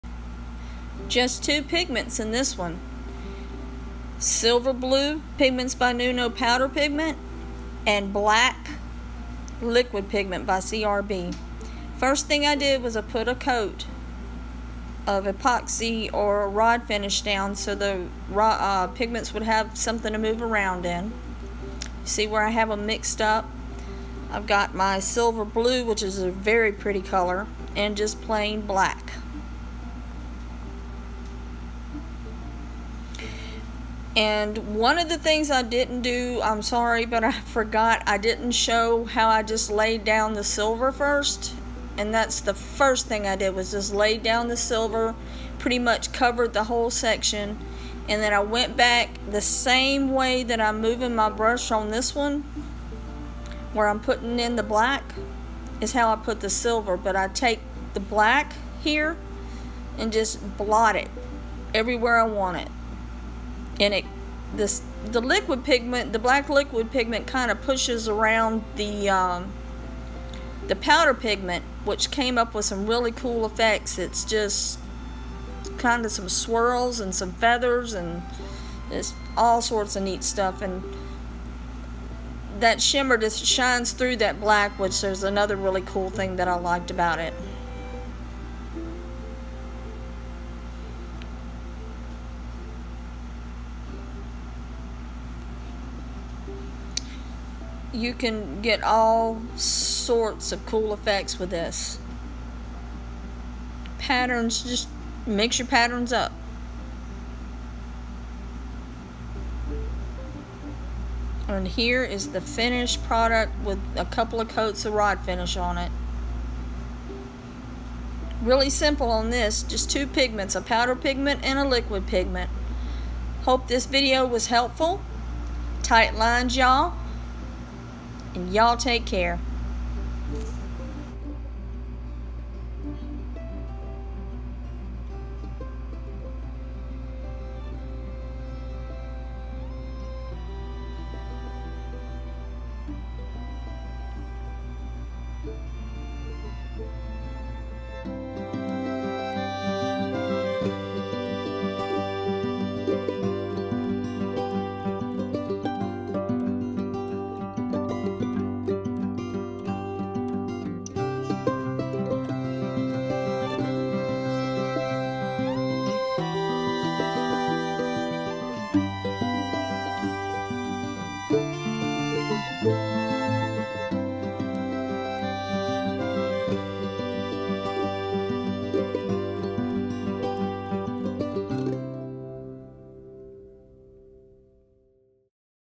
Marbling Just 2
Custom Rod Building with custom marbling. Just 2 pigments CRB Black liquid pigment and Pigments by Nuno Silver Blue